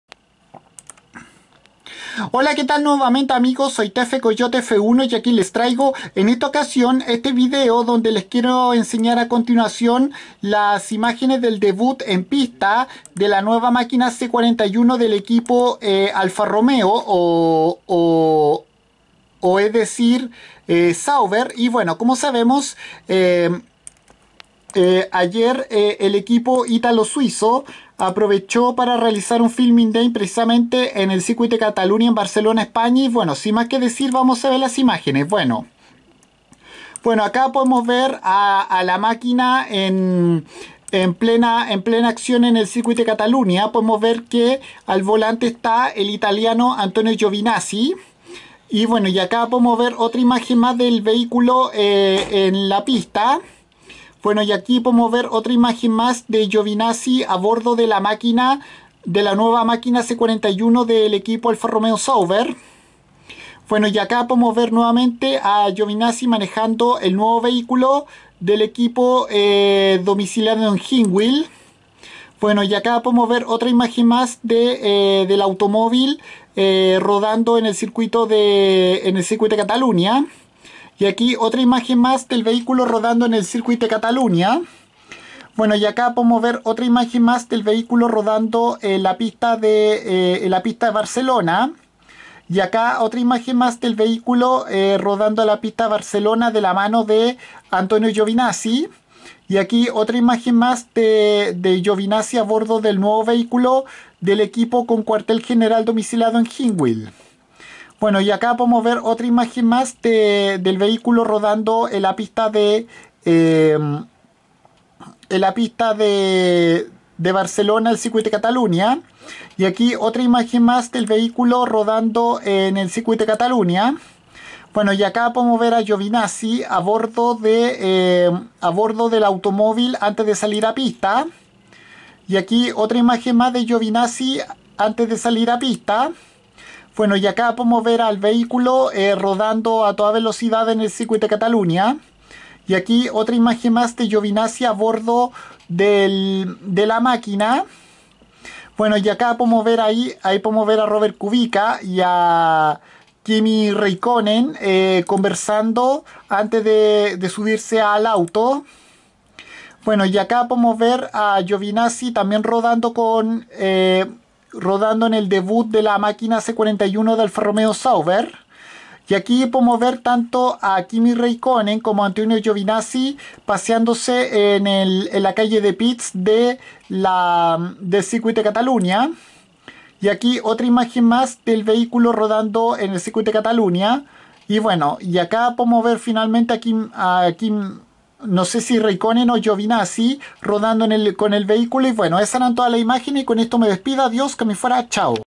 Las primeras fotos en pista del F1 Alfa Romeo C41
El conjunto ítalo-suizo tuvo un filming day en el Circuit de Catalunya en Barcelona España